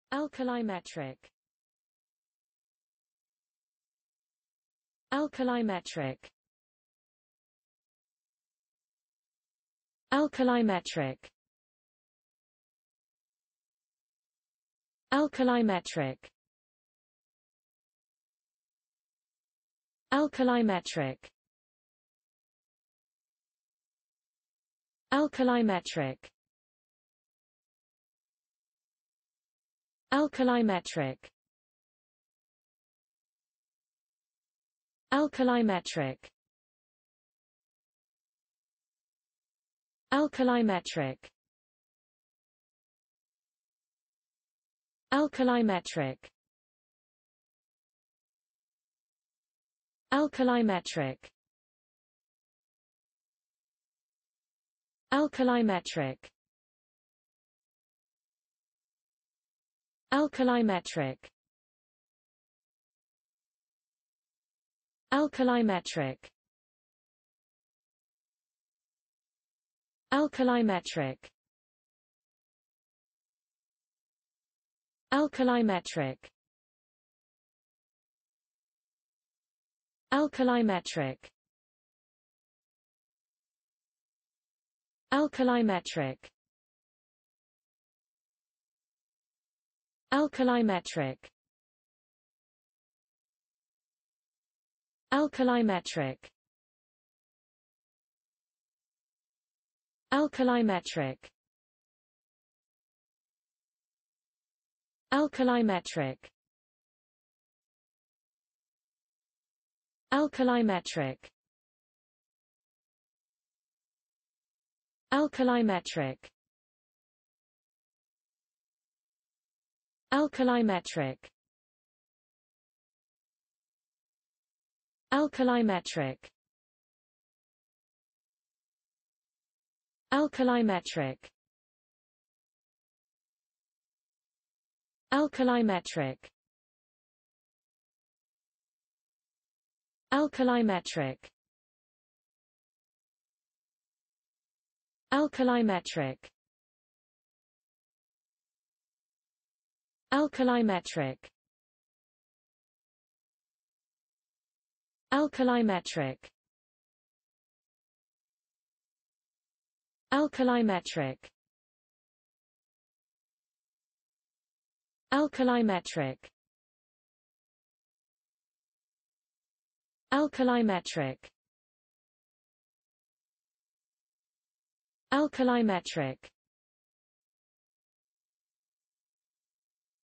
alkalimetric: How to pronounce alkalimetric in english (correct!).Start with A. Learn from me.
Follow me!Learn from me how to pronounce the word alkalimetric in english.This video shows you how to pronounce the word alkalimetric in english.

alkalimetric

碱量测定的